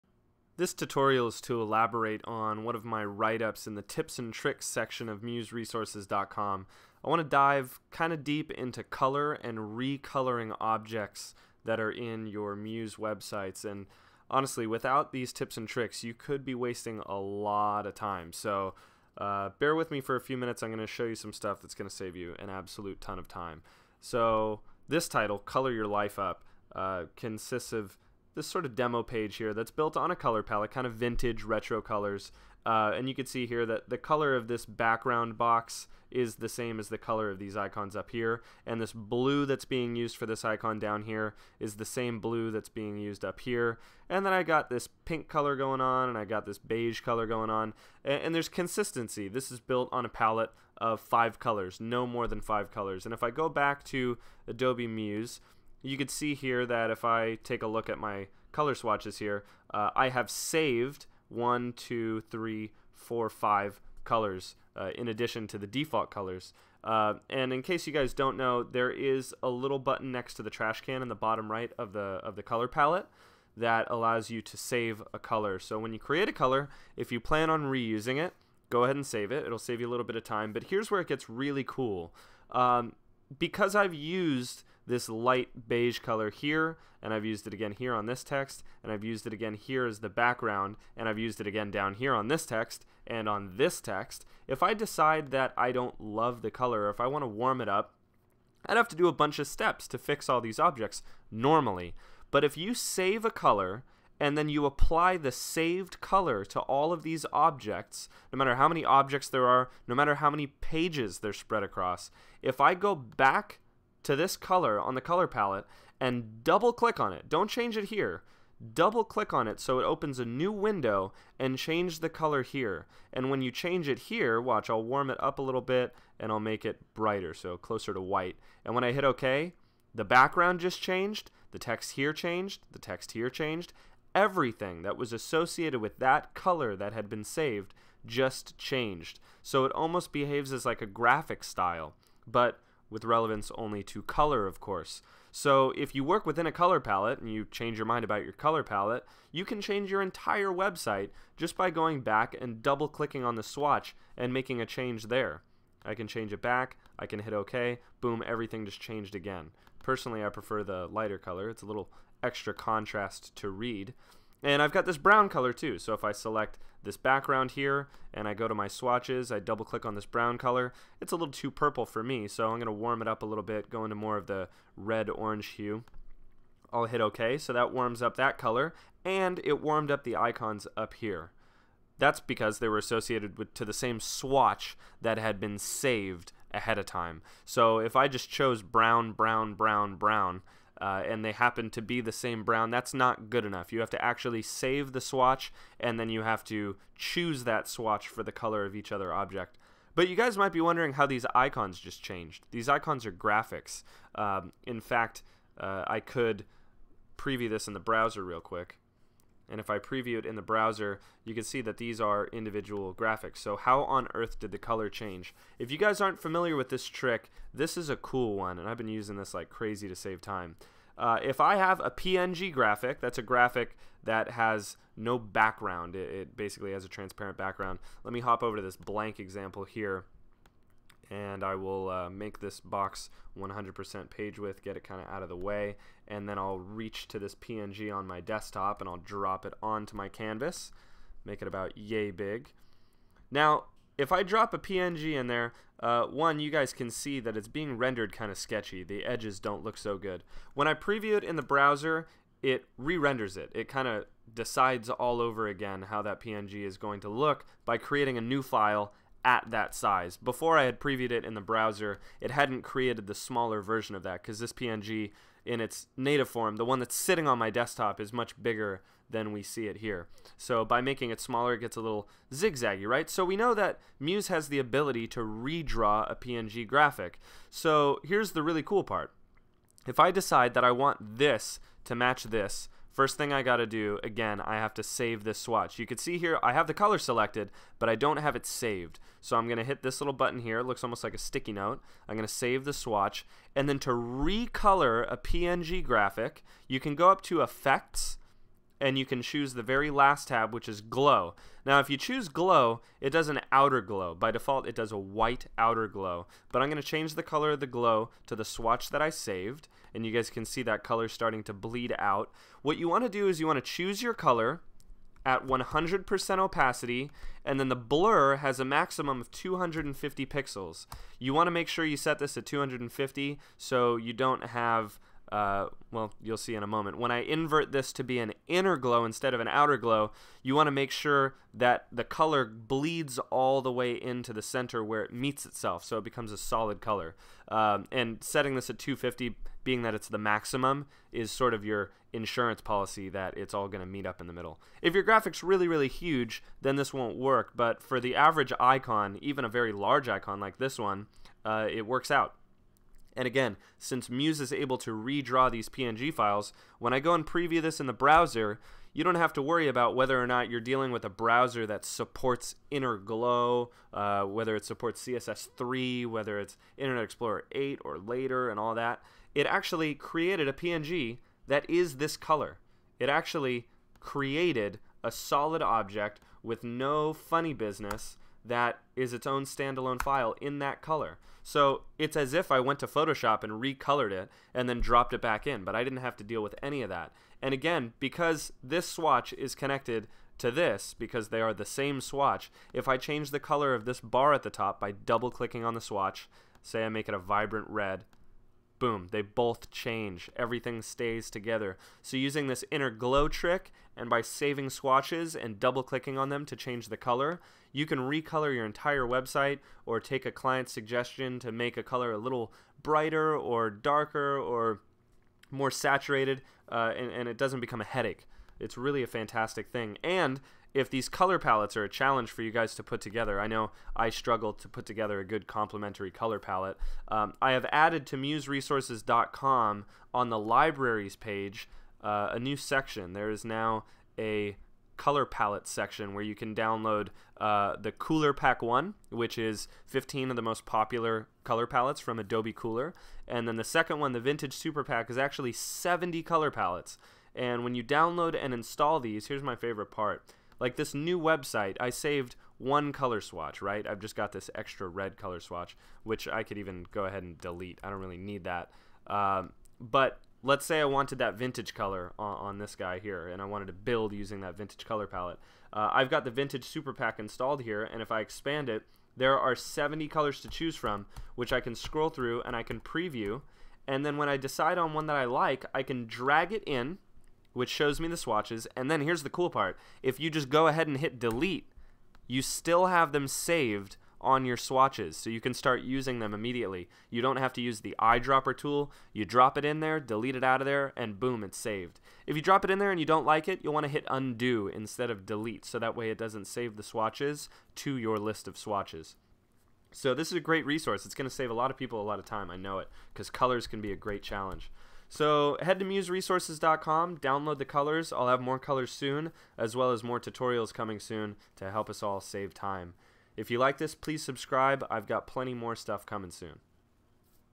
Adobe Muse CC 7.0 Tutorial | Color Your Life Up
We designers hate when our brain does one simple thing but our hands have to do 30 things to make it happen. We use and reuse colors in our website designs. Often we use the same color for over a hundred objects then fear the possibility of having to change that color. In this tutorial I show you how saving and changing color swatches can save you a ton of time. Also covered is changing the color of PNG graphics within Adobe Muse CC without the need for Photoshop or other graphics editing software.

With the tricks in this tutorial, color will never be a challenge again.